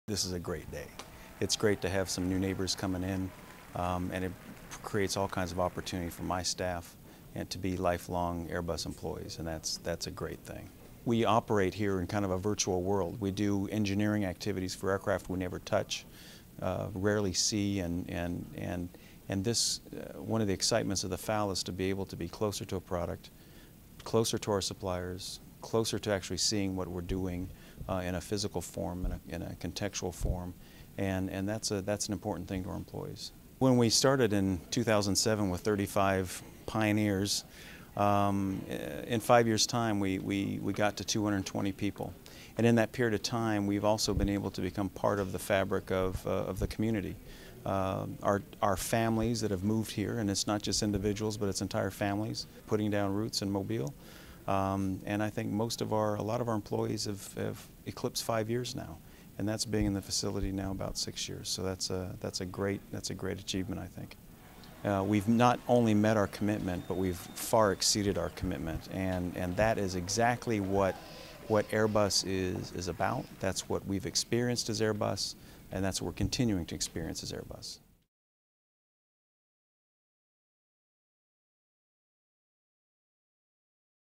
Assembly line Mobile: David Trent interview
David Trent, the Airbus Mobile Engineering Center's site director, shares his thoughts on the A320 Family final assembly line to be built in Mobile, Alabama, and describes how Airbus already has become "part of the fabric" in the area.